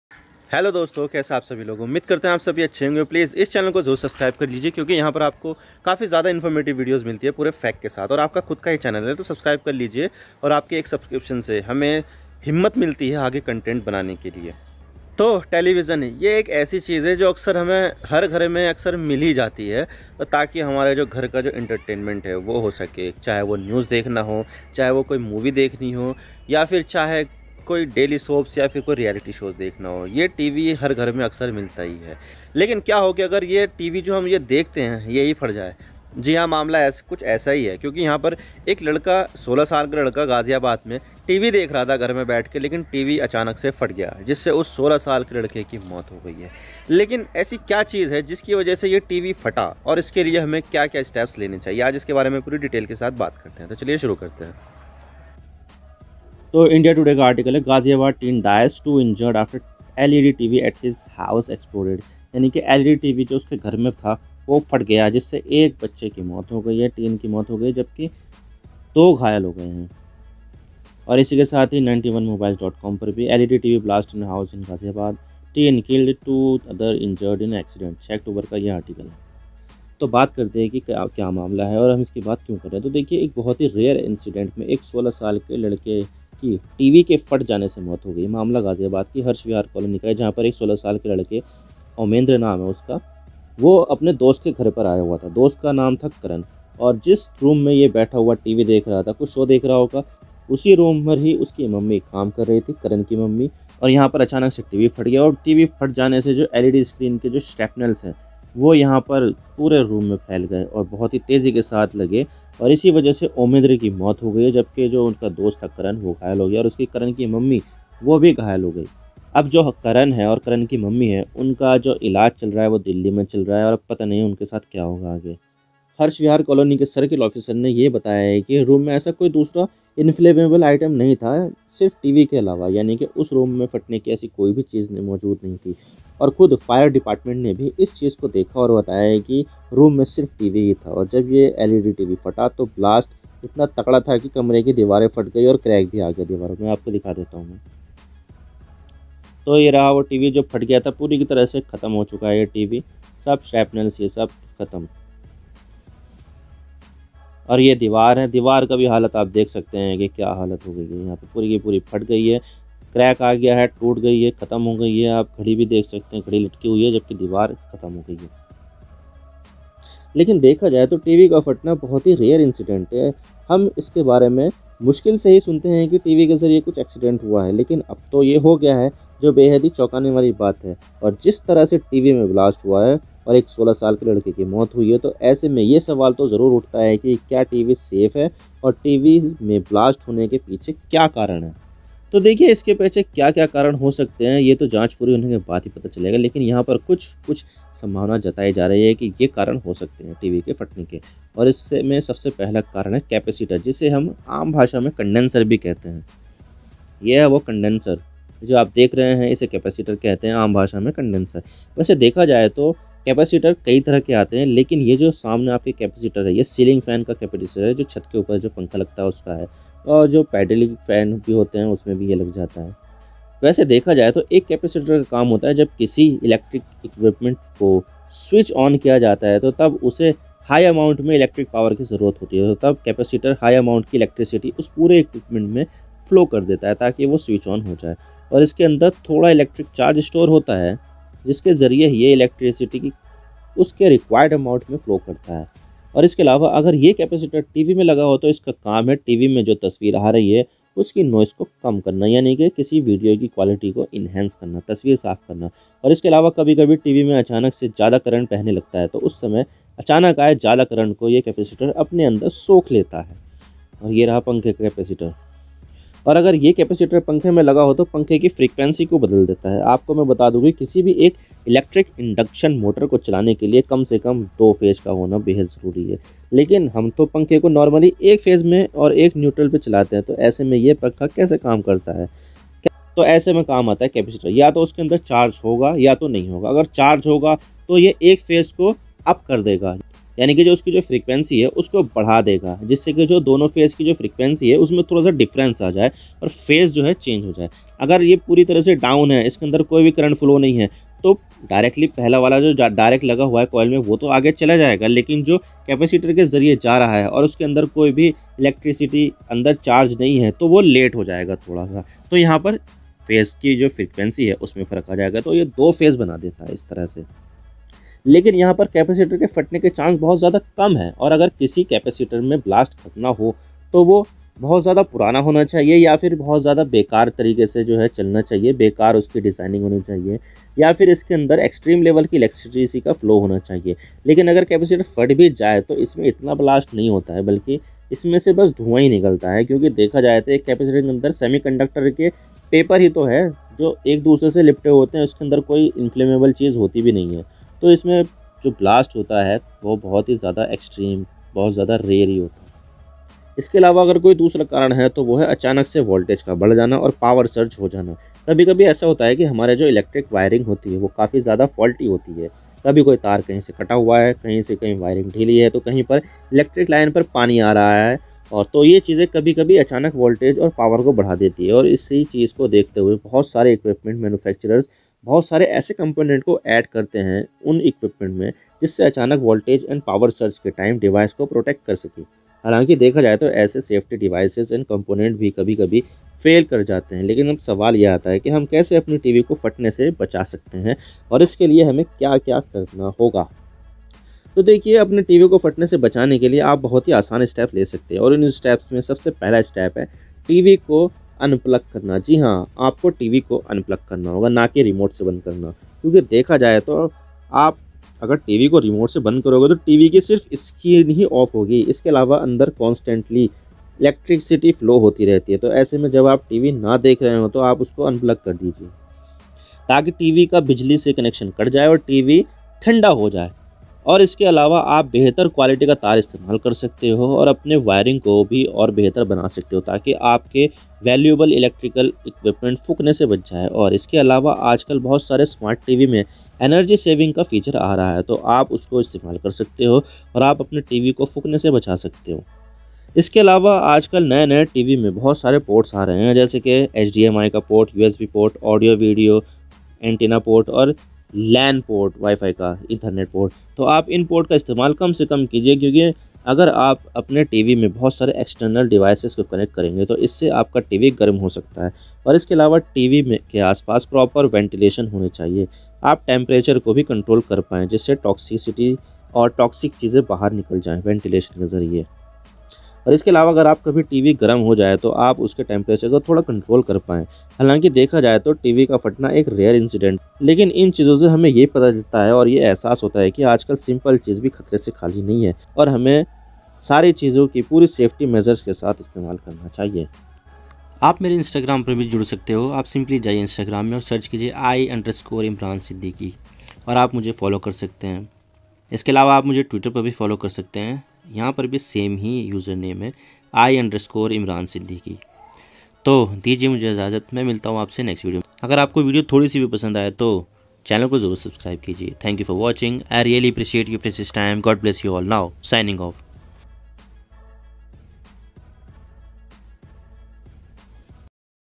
One Killed in TV Explosion in Ghaziabad | Why TV Exploded? | How to Prevent Explosion? | InfoMagnet
Hi Friends, In this video We will talk about one killed in tb Explosion in ghaziabad. how did tv Exploded in ghaziabad? how to prevent Explosion? let's see

Host: Imran Siddiqui
Voice: Imran Siddiqui
Edited By: Imran Siddiqui

All Advice, Suggestion, Criticism and Appreciation Are Welcome